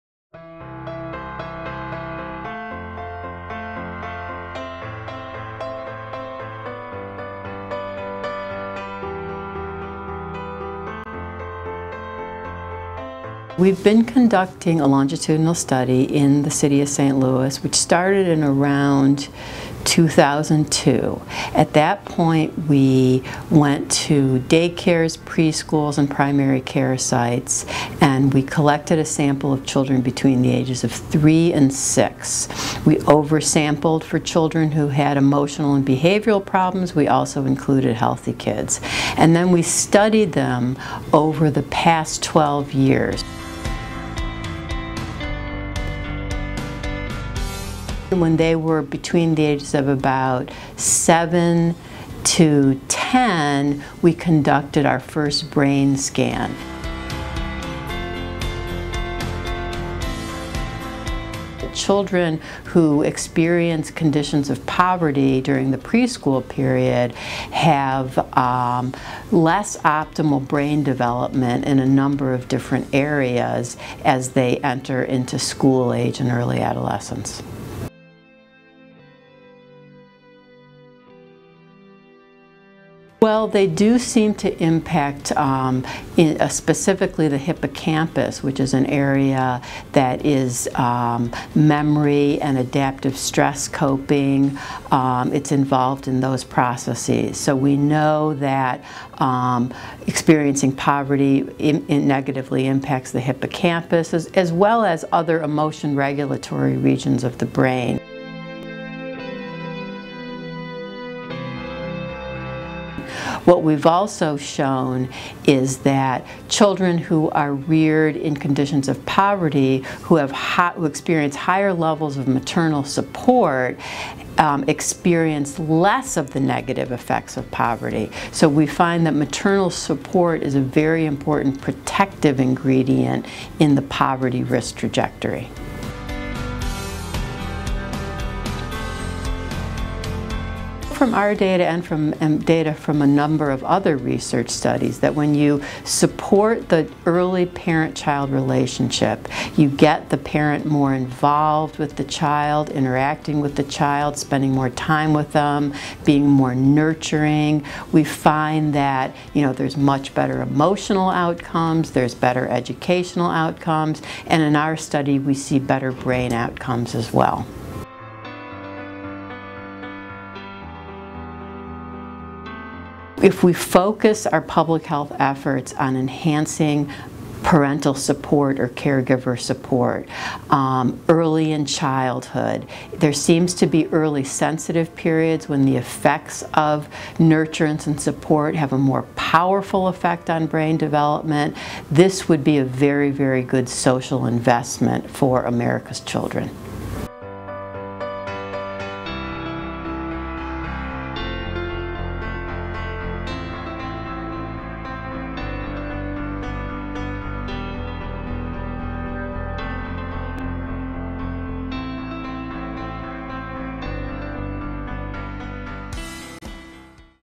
Dr. Joan Luby talks about the effects of toxic stress on children
Washington University School of Medicine psychiatrist and researcher Joan Luby to discuss her long-term research findings on the effects of poverty and toxic stress on brain development of children.